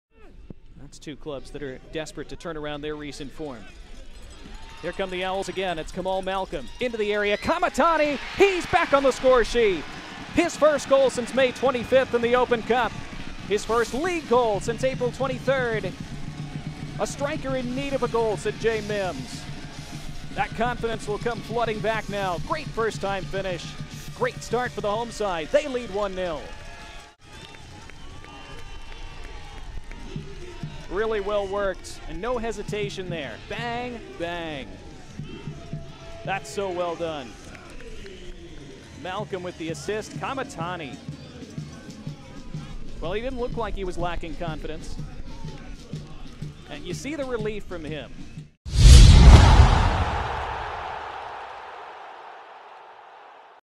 Hugo Kametani with a Goal vs. FC Tucson
Hugo Kametani (Union Omaha) with a Goal vs. FC Tucson, 07/16/2022